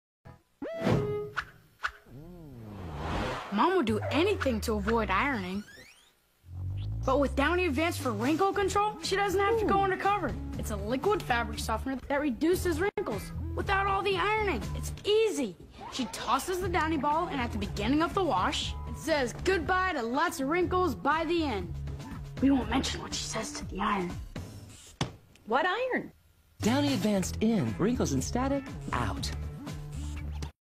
Downy Fabric Softener Commercial 2003
Downy is a brand name of fabric softener produced by Procter & Gamble and sold in the United States. It entered the U.S. test market in August 1960 and went nationwide in December 1961. It was also sold in the Philippines, Malaysia, Indonesia, Singapore, Thailand, Vietnam, South Korea, Egypt, Kenya and Latin America.
Lenor is a brand name of fabric softener and dryer sheets, also produced by Procter & Gamble, sold in Europe, Russia, China and Japan. Plans to rebrand Lenor as Downy in the UK were dropped in 2002.
Amy Sedaris has been used to promote Downy/Lenor Unstoppables range in both United States and the UK. The commercials were filmed by Grey Advertising.